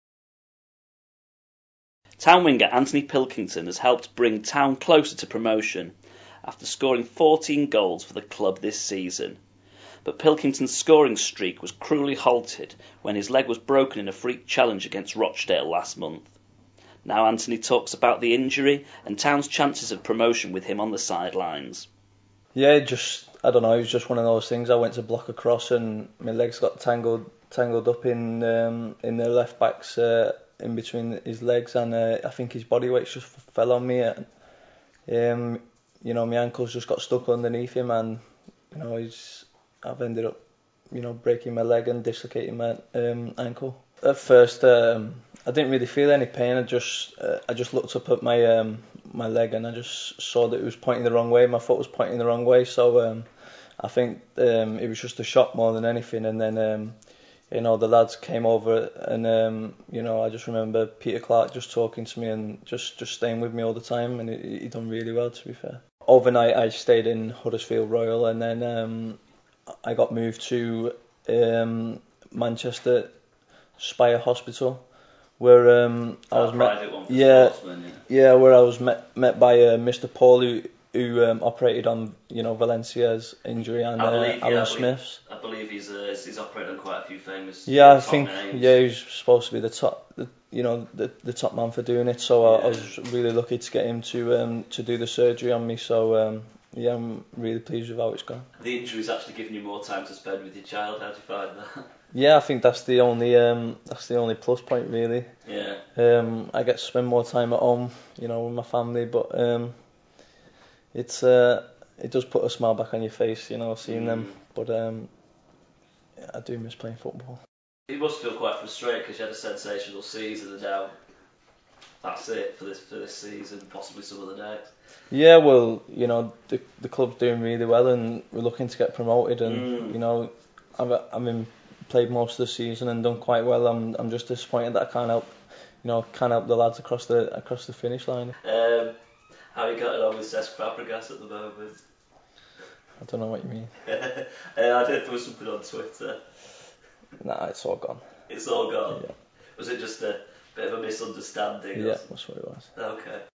Huddersfield Town's Anthony Pilkington on football, family and Arsenal star Cesc Fabregas
Huddersfield Town star Anthony Pilkington explains how his injury happened, his hopes for Huddersfield Town's season and more.